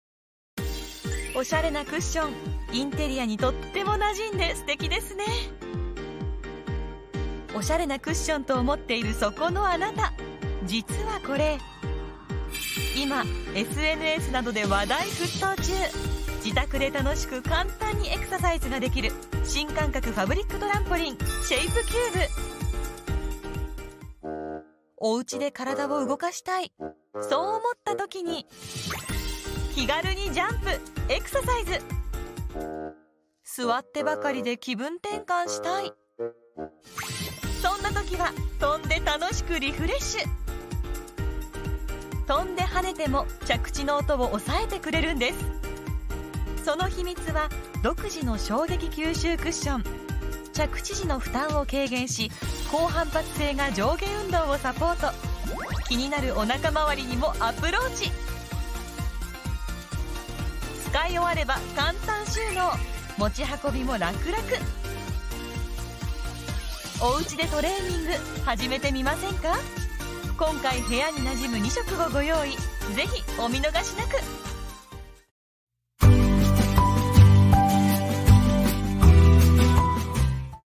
有酸素運動＆筋トレ！お部屋に溶け込みすぎるおしゃれなデザインも魅力的なファブリックトランポリンクッション【シェイプキューブ】
動画で紹介している商品を見る

※動画の商品は販売終了の可能性があります

【美容・健康アイテム・家電】関連動画をみる

ショップチャンネル　ホームページはこちら

１．【商品概要】
・まるでクッションのような見た目でありながら、本格的なジャンプ運動ができるファブリックトランポリン「シェイプキューブ」のご紹介です。
・スプリングのような弾力性のある反発素材（トランポリンファイバー）と、着地時に足や腰、膝への衝撃を抑える素材（衝撃吸収クッション）を組み合わせることで、跳びやすくしながらも、飛びすぎないよう反発を抑えつつ衝撃を吸収する設計。さらに裏側には滑り止めをほどこしました。
・お部屋に自然となじむおしゃれなデザインも魅力。片手でらくらく持ち運びできるので、いつでも好きな時に好きな部屋で運動できます。→２０２０年グッドデザイン賞受賞。
２．【シェイプキューブ】
■構造：
・それぞれの素材を活かした４層構造の設計になっています。衝撃吸収層と反発層を組み合わせることで、跳びやすく、衝撃や振動をしっかりと抑えます。
・１層目は、足、膝、腰への衝撃を吸収して和らげる、衝撃吸収素材。ジャンプ運動時に跳びすぎないよう高さを抑えつつ、着地時には衝撃吸収素材の働きにより、何もない床の上でジャンプするよりも、ジャンプ運動中の足・腰・膝に負担が掛かりにくくなっています。また、振動や騒音を吸収して響きにくくするため、何もない床の上でジャンプするよりも、楽しいジャンプ運動が気兼ねなくできそうです。
・２層目から４層目は、反発性の高い素材を組み合わせて使用しました。飛び上がりやすく、しっかりと跳ぶことができます。
■使い勝手の良い仕様：
・本格的なハードトレーニング、バランス運動トレーニング、姿勢トレーニング、踏み台昇降トレーニングなど、ジャンプ運動以外の使用用途もあるため、子供からご年配の方までそれぞれの状況に合わせてお使いいただけます。
・見た目がとてもおしゃれで運動器具とわかりにくく、ご自宅のどの部屋に置いてもなじみやすいのもポイント。ちょっとしたクッションや座布団替わりとしても使用可能です。
・コンパクトサイズで、組み立てる必要もないので便利。持ち手が付いているため、移動や設置、収納がスムーズに行えそう。
・アウターカバーは、取り外して手洗いできます。
→３．【ブランド情報】
・数々の健康アイテムをプロデュースする“アメイズプラス”から、２０２０年秋に誕生したブランドが“ジムテリア（Ｇｙｍｔｅｒｉｏｒ）”。※ジムテリア：ジム（Ｇｙｍ）＋インテリア（Ｉｎｔｅｒｉｏｒ）
・これまでにない洗練されたデザインと、エクササイズアイテムとしての高い機能性を両立し、家族で、オフィスで、スマートに健康で豊かな毎日を送るためのブランドです。お部屋に自然となじむ、ホームエクササイズアイテムを提供しています。